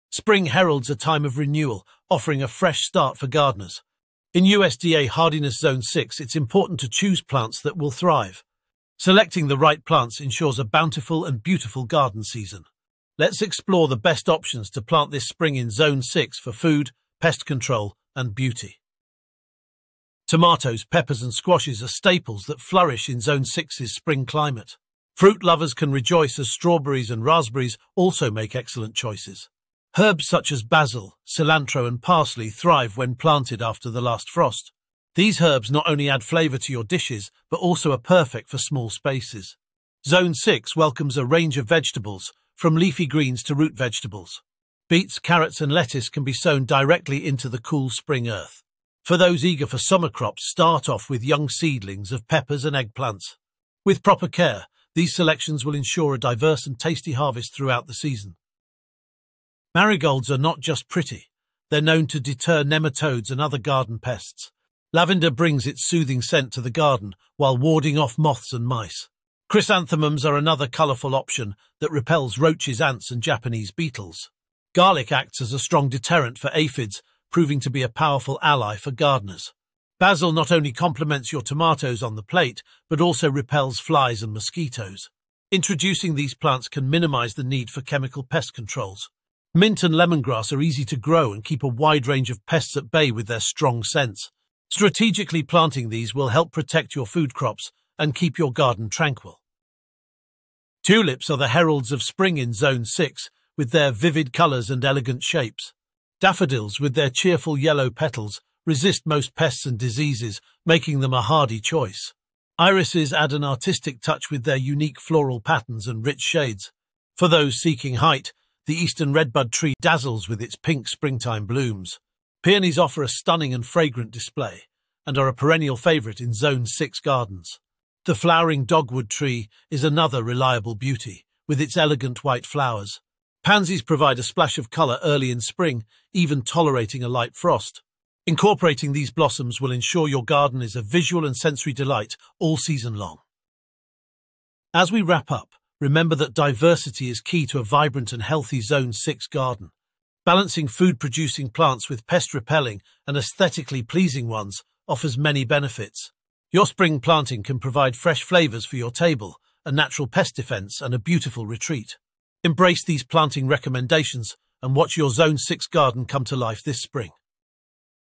Grow These Spring Plants in Zone 6 for Beauty & Bounty!
Spring has sprung in Zone 6, and it’s the ideal time to breathe new life into your garden. Understanding the importance of selecting the right plants for your Zone 6 garden ensures not only the beauty and bounty of your outdoor space but also its resilience and sustainability. From vibrant vegetables to fragrant herbs, there are countless options to cultivate a garden that promises a fruitful season. Whether you're aiming for a table laden with home-grown produce or simply love the serenity of a green space, starting with the right foundation is key. Get ready to transform your garden into a haven of productivity and enchantment this spring.

Dive into the world of food-providing greens, pest-deterring flora, and eye-catching blooms perfect for Zone 6 planting. Discover the top vegetables and fruits that will thrive in your garden alongside herbs that add both flavor and freshness to your spring planting endeavors. Learn about the magic of companion planting with plants that naturally ward off unwanted garden pests, ensuring the health and prosperity of your garden. Furthermore, explore beautiful flowering plants and ornamental shrubs that not only enhance the aesthetics of your space but also boost its biodiversity. With a thoughtful selection of plants, achieve a garden that is not only thriving and productive but also a reflection of your personal taste and a haven for local wildlife. Embrace the variety and let your Zone 6 garden flourish this spring.